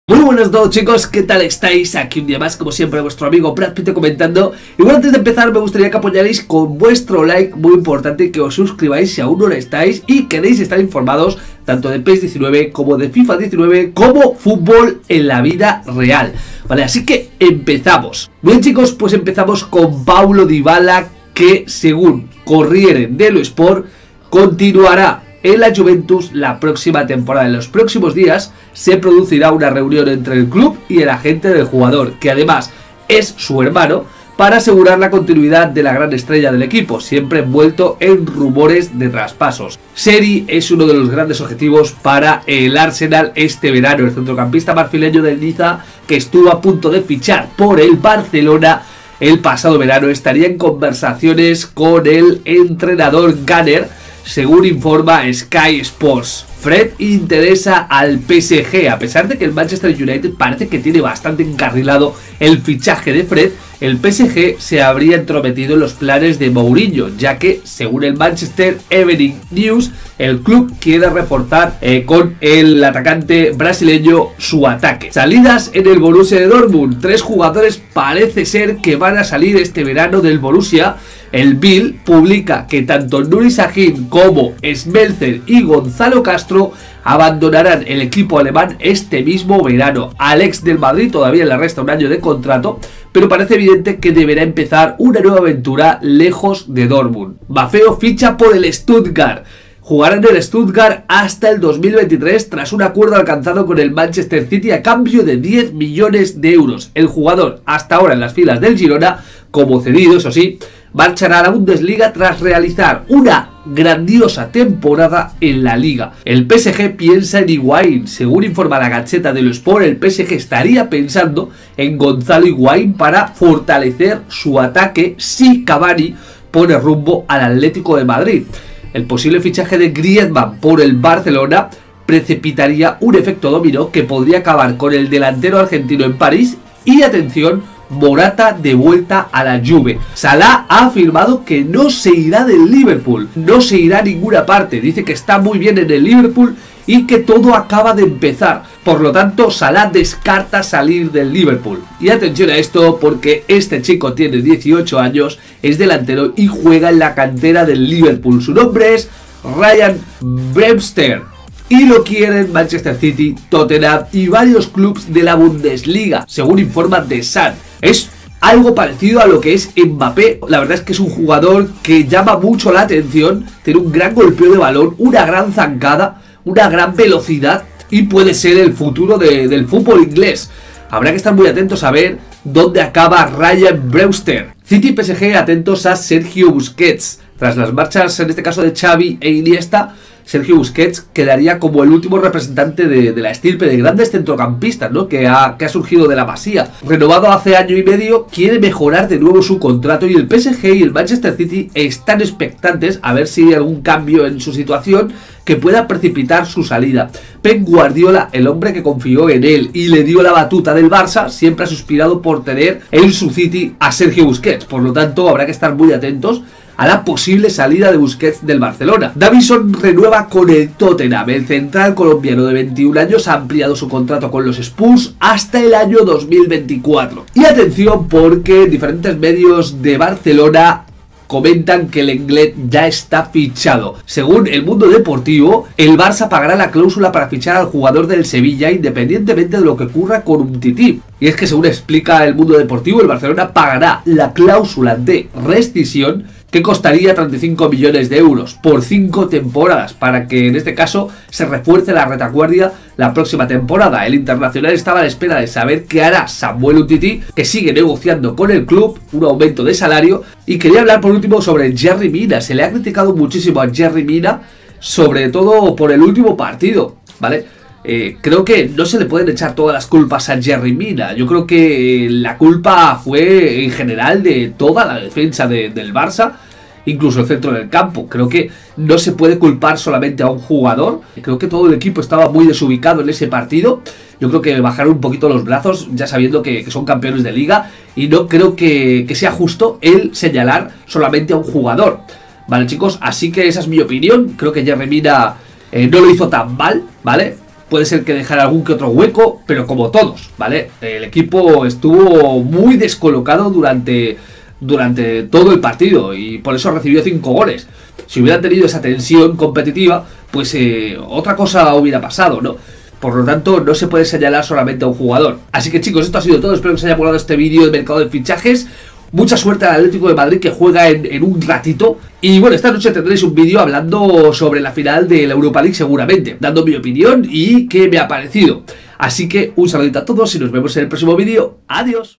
MERCADO DE FICHAJES 2019 CONFIRMADOS y rumores! Lenglet, Higuain, Busquets, Yerry Mina y más!
Mercado de fichajes 2018/2019, el mercado de fichajes de verano ya se mueve, Lenglet será jugador del Barcelona, Salah esta a gusto en el liverpool y no contempla un cambio de aires en este caso al Real Madrid, Yerry Mina se podría ir en este mercado de verano, suena el arsenal.

Que ganas tengo de iniciar un modo carrera en fifa 19, estoy impaciente por ver las novedades que traerá el modo carrera, por supuesto también de pes 2019, donde espero que la liga master y ser una leyenda sufran cambios enormes.